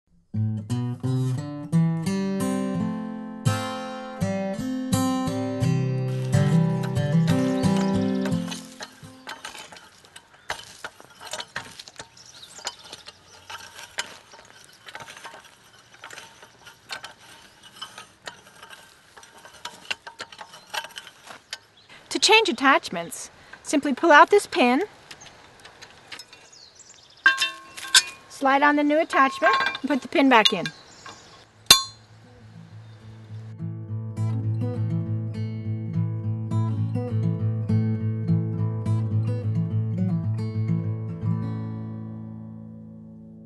Valley Oak Tined Cultivator Attachment
The Valley Oak tined cultivator attachment in action. This attachment is great for breaking up soil and working in soil amendments. The Valley Oak wheel hoe is a fantastic tool for large gardens and small farms.